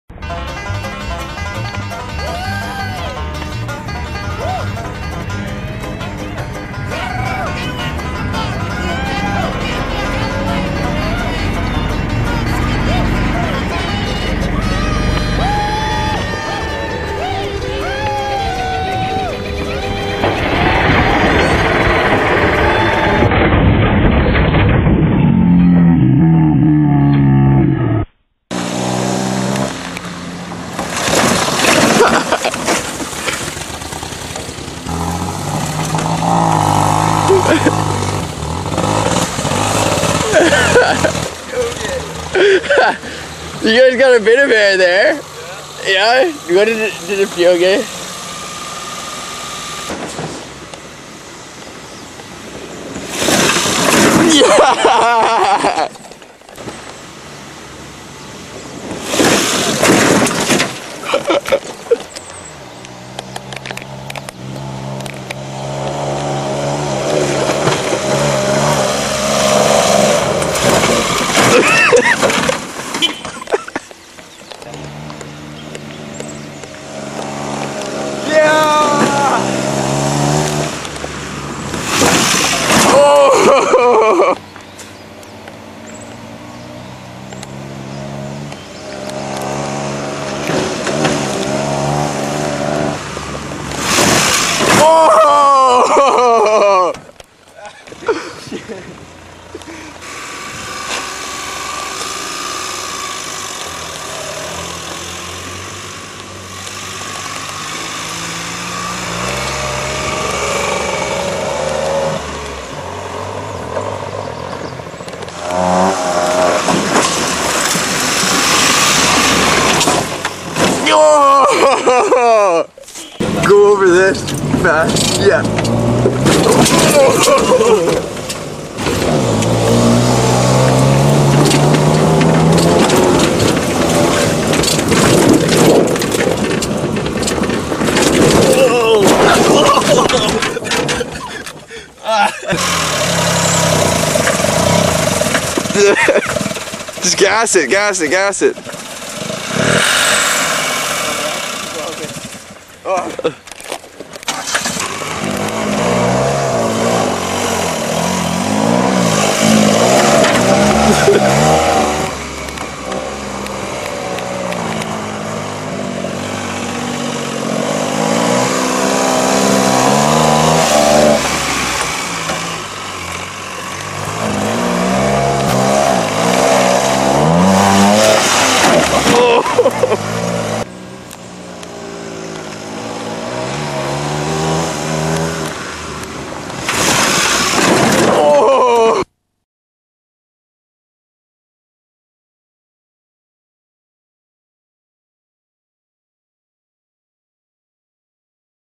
Jumping Chevy Sprint
KILL YOUR CAR!  After a long summer of constant overheating, and after a 14 day inspection notice from saanich police, my old chevy sprint finally sh*t the bed. So i limped it over to my buddy Bens house where we finished her off in a festive manner. Enjoy!